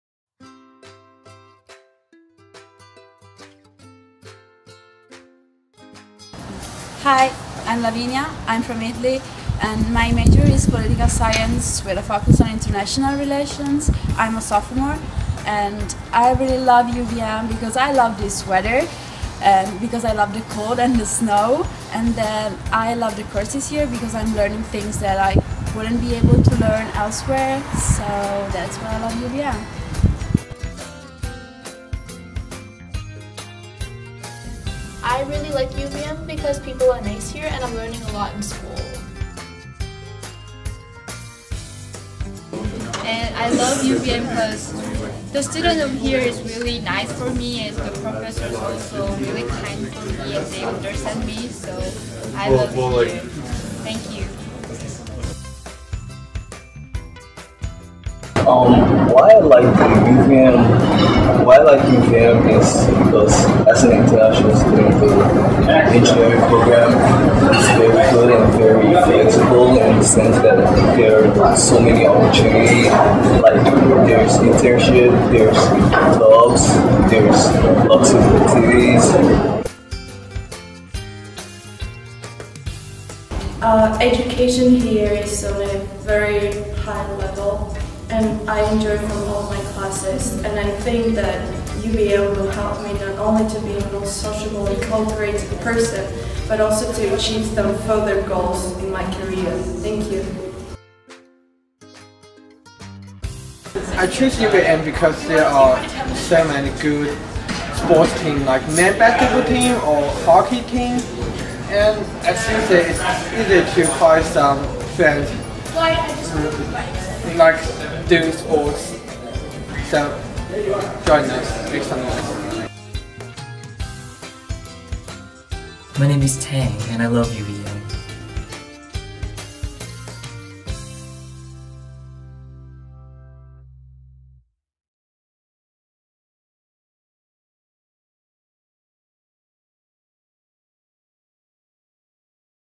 UVM Love from Around the World
International students share their love for the University of Vermont.